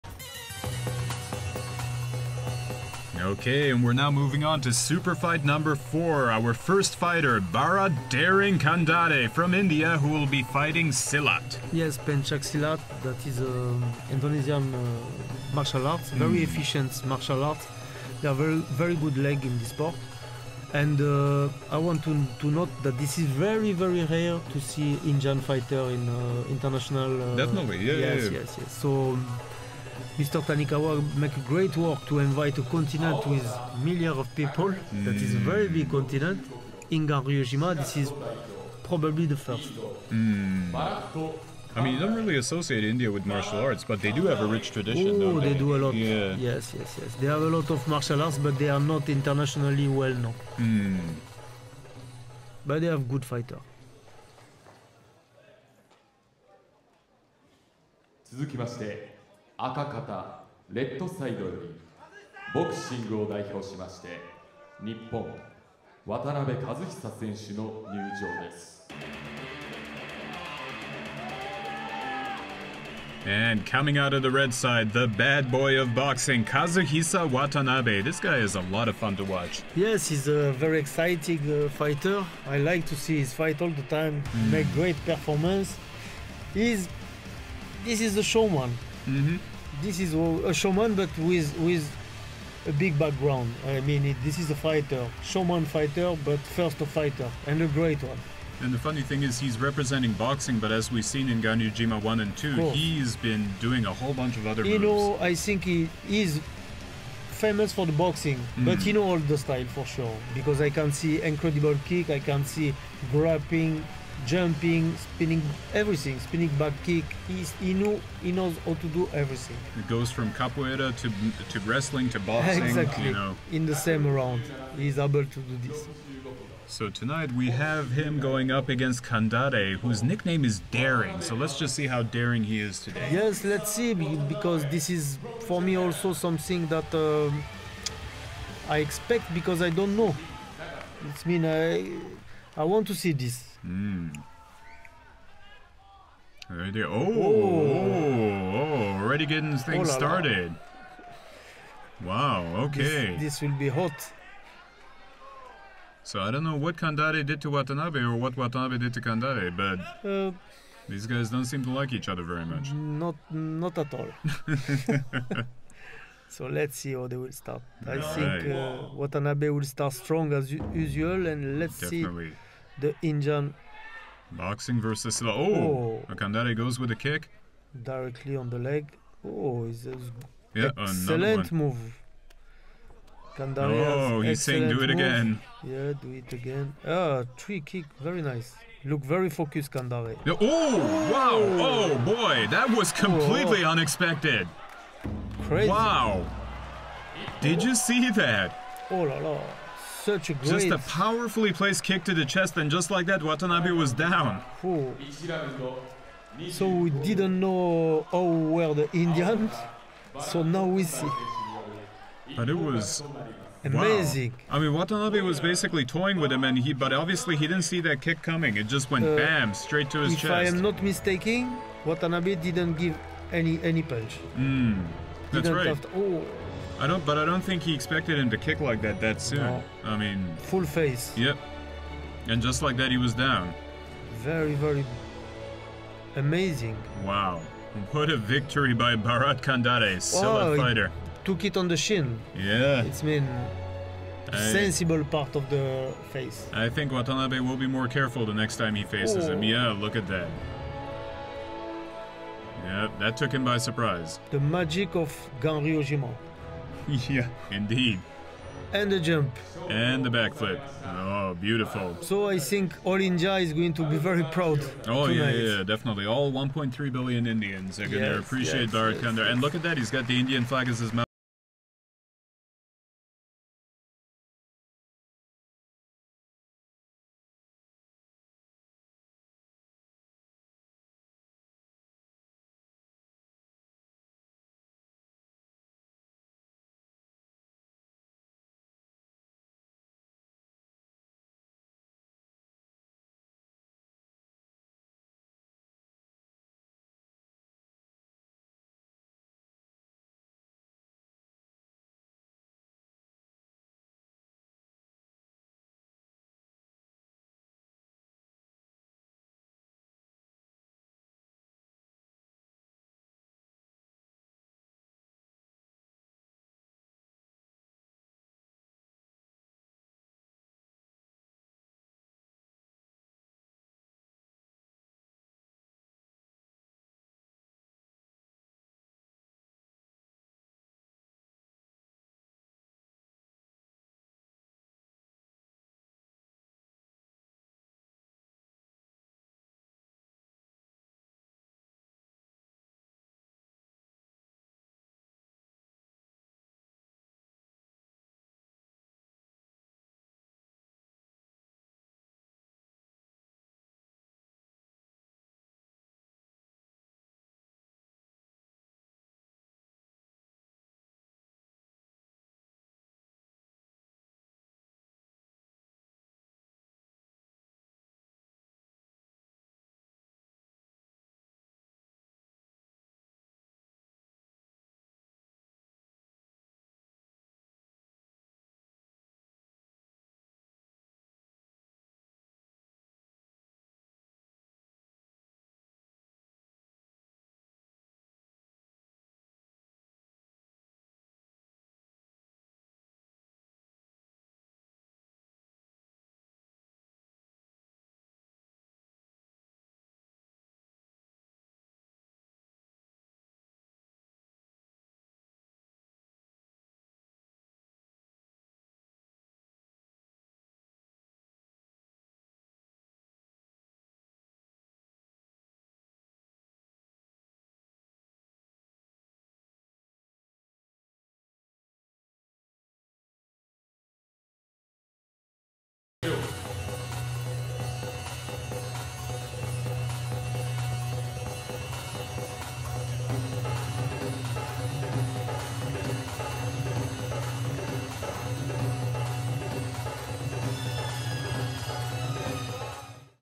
【ボクシング × シラット】渡辺一久 vs バラット・カンダレ - Kazuhisa Watanabe vs. Bharat Kandare【巌流島】
巌流島 STARTING OVER 2016
【ボクシング × シラット】渡辺一久 vs バラット・カンダレ
【Boxing × Silat】Kazuhisa Watanabe vs. Bharat Kandare